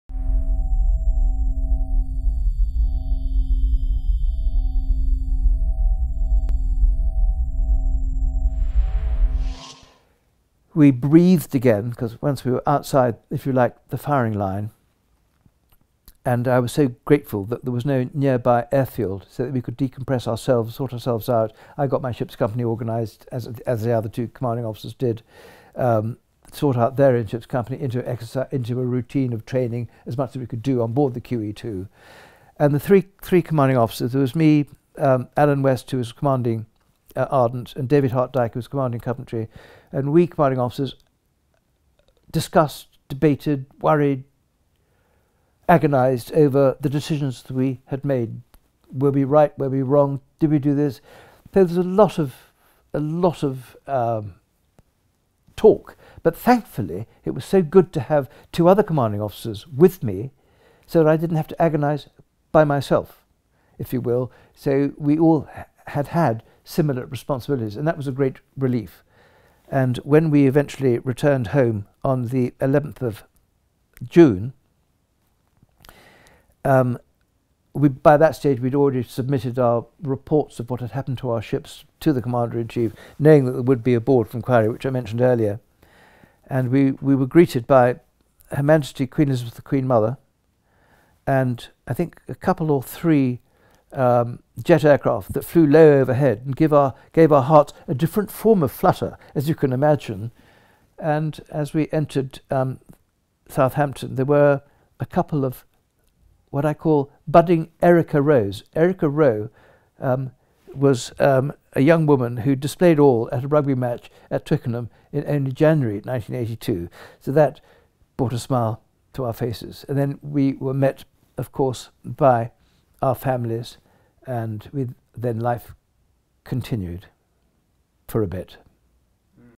FALKLANDS WAR | Sinking Of HMS Antelope. Nick Tobin Reflects On The Day His Ship Was Sunk.
The Falklands War Sinking Of HMS Antelope. Captain Nick Tobin Reflects On The Day His Ship Was Sunk. FalklandsWar Antelope SanCarlos Time Stamp Below

HMS Antelope took part in the Falklands war, arriving in the area of operations on 21 May 1982. Two days later, while on air defence duty at the entrance to San Carlos Water, protecting the beachhead established two days before, she came under attack by four Argentine A-4B Skyhawks of Grupo 5. The first pair attacked from astern, with the flight leader breaking off his attack after one of Antelope's SeaCat missiles exploded under the port wing of his aircraft.

Time Stamp
00:00 Intro
00:23 Re-org Gathering the Ships Company
00:30 Reflecting On The Attack
01:05 Returning Home on June 11th
01:20 Being Greeted By The Queen Mother

Nick assumed command of the frigate HMS Antelope in early 1981 and, during the campaign to recover the Falklands after the Argentines invaded the islands in 1982, his ship was bombed and later sank in San Carlos Water on 23 May. On his return from the South Atlantic, he served first as a Staff Officer in the Naval Staff and then as the Military Assistant to the Vice Chief of Defence Staff in the Ministry of Defence. Following his promotion to Captain in 1986, he was accredited to Japan as the Defence and Naval Attaché, based in the British Embassy in Tokyo. Returning to London in 1990, he headed the Warfare Branch Development Team where he masterminded a major shift in the way the Royal Navy trained and employed its service personnel. In 1994, following 18 months as the Director of Naval Manning and Training (Seaman), he was selected to set up and head the key Directorate of Naval Manning in London and Portsmouth.

FOR EVERYTHING FROM LEGASEE WAR VETERANS

After initial damage control efforts, Antelope proceeded to more sheltered waters so that two bomb disposal technicians from the RoyalEngineers could come aboard and attempt to defuse the two unexploded bombs. One of the bombs was inaccessible because of wreckage; the other had been damaged and was thought to be in a particularly dangerous condition. Three attempts by the bomb disposal team to withdraw the fuse of this bomb by remote means failed.
A fourth attempt using a small explosive charge detonated the bomb, killing Staff Sergeant James Prescott instantly and severely injuring Warrant Officer Phillips, the other member of the bomb disposal team.

The ship was torn open from waterline to funnel, with the blast starting major fires in both engine rooms, which spread very quickly. The starboard fire main was fractured, the ship lost all electrical power, and the commanding officer, Commander Nick Tobin, gave the order to abandon ship. Tobin was the last person to leave the ship; about five minutes after his departure, the missile magazines began exploding.